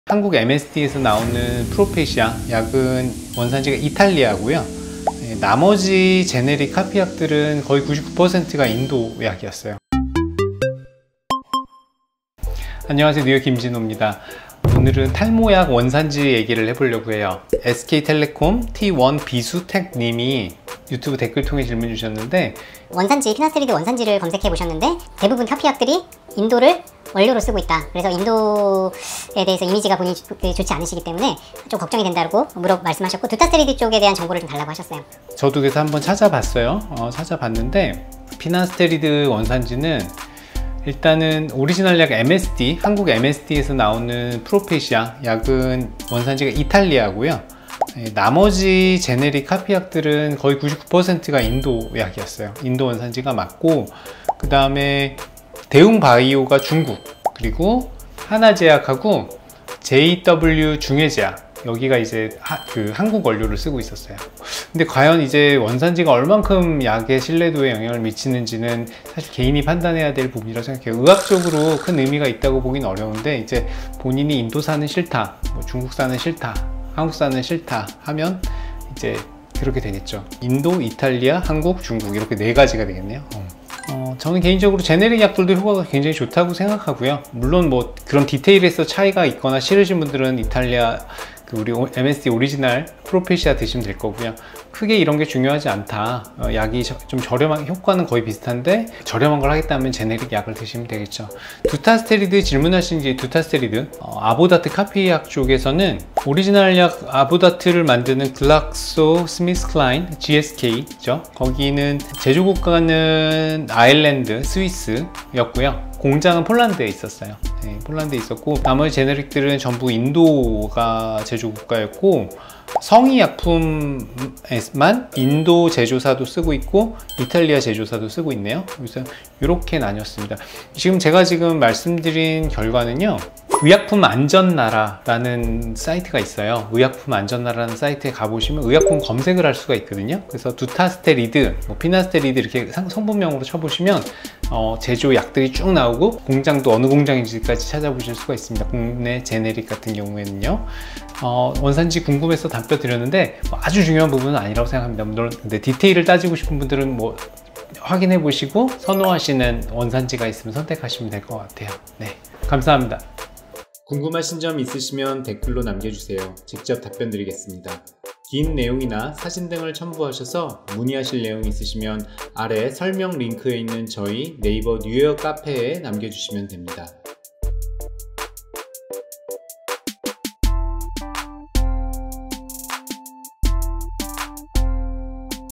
오리지널 탈모약과 카피약의 원산지?
탈모 카피약들의 원산지가 대부분 인도던데.. 믿을 수 있는건가요?
원산지에 따라서 탈모약의 효과도 다를까봐 걱정이 됩니다..!
피나스테리드, 두타스테리드의 원산지 알려주세요!📢

📁탈모약 박사가 되는 길